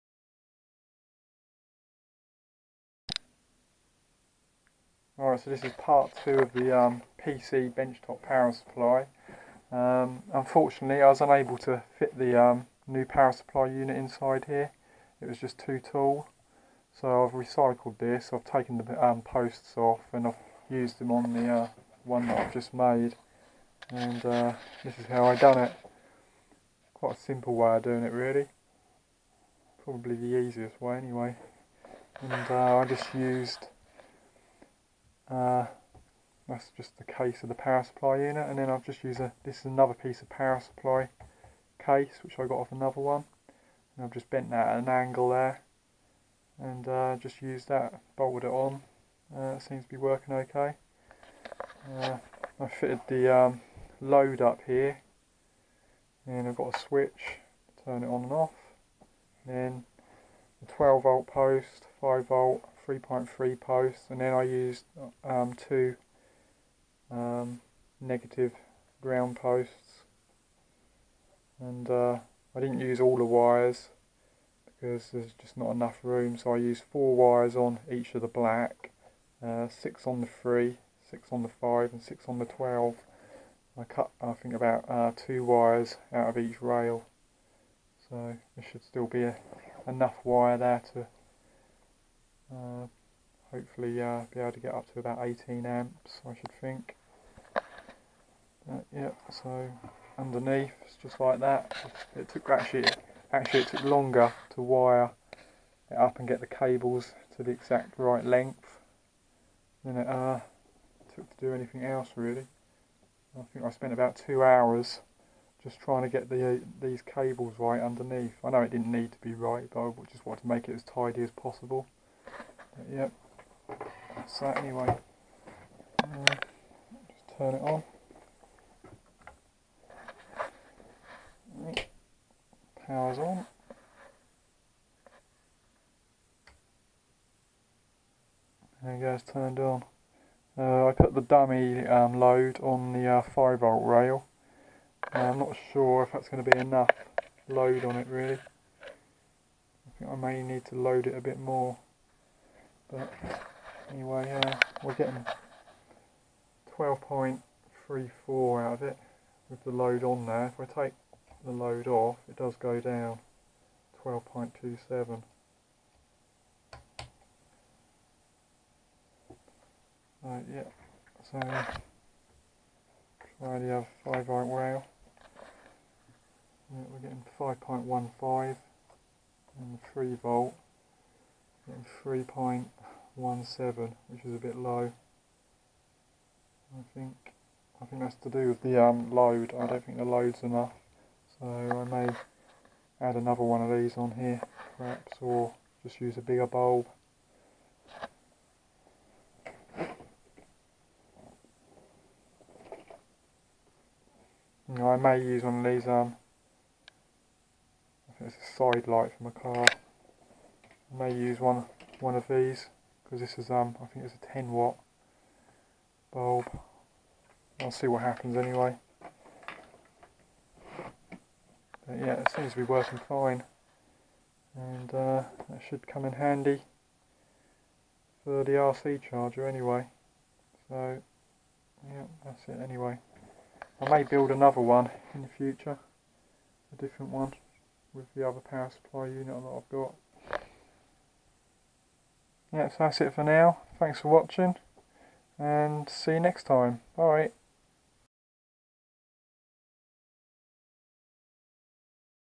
Converting Computer Power Supply to Bench Supply Pt 2
Built my new bench power supply for charging rc battery's Converting Computer ATX Power Supply to Lab Bench Power Supply